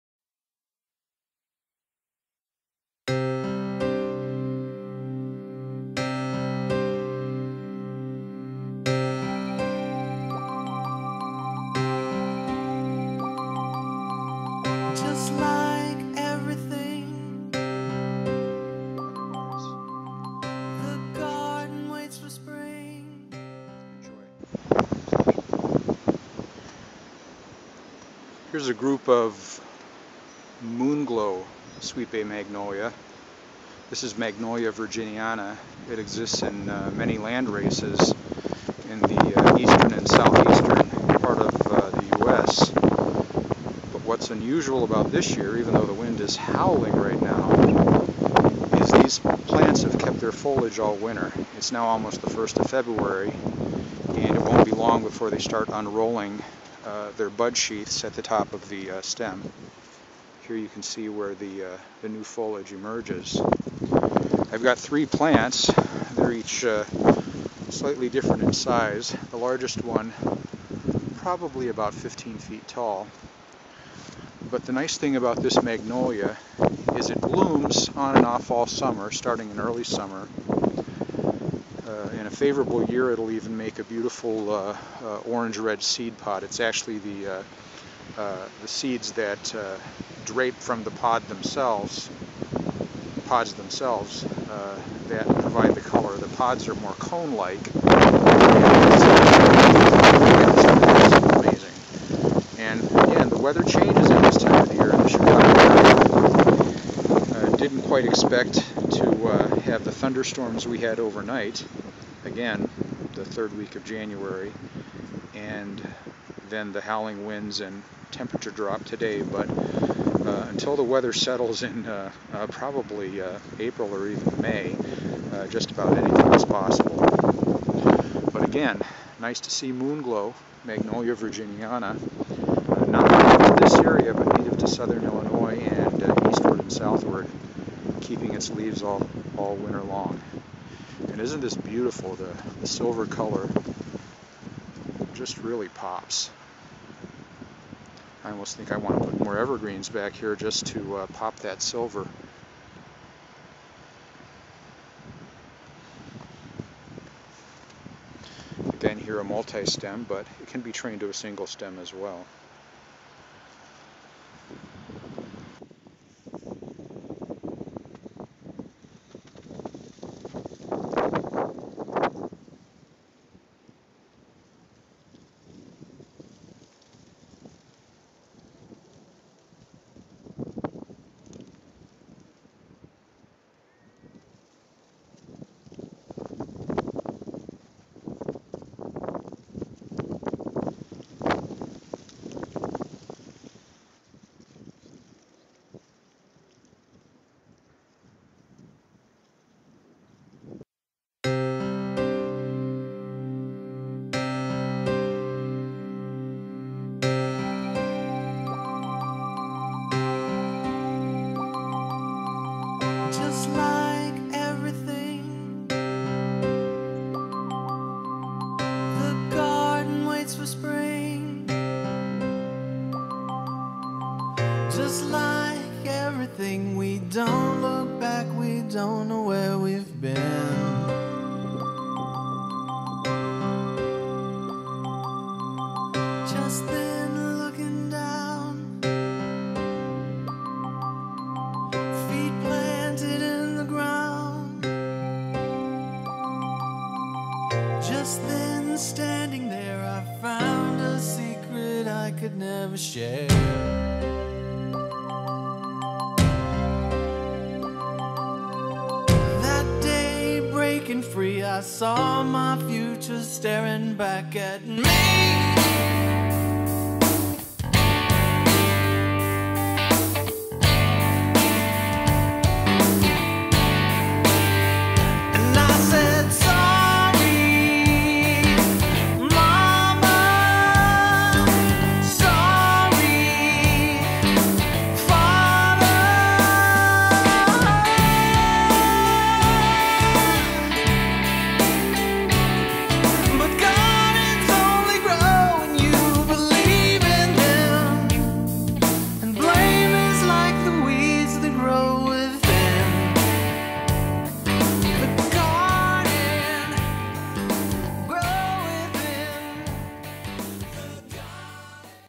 Garden Opus - Before Spring Awakens - Magnolia Virginiana 'Moonglow'
In HD.  A 5 minute "short take" from "Before Spring Awakens Part 1" -- in the face of howling late January winds and alternate freeze-thaw cycles, Magnolia Virginiana 'Moonglow' -- a Sweet Bay Magnolia selection well adapted to the upper midwest -- is admired for its winter leaf retention, silver-backed foliage, early summer flowers and ornamental seed structures.  There are many landraces of this Magnolia native to the east and southeast US, and the concept of landrace is briefly described.

With Award-Winning Horticulturist, Hybridist, Author, Consultant and Photographer Robert F. Gabella, filmed in suburban Chicago in late January, 2012.  Features original music by David (Ray) Stephens.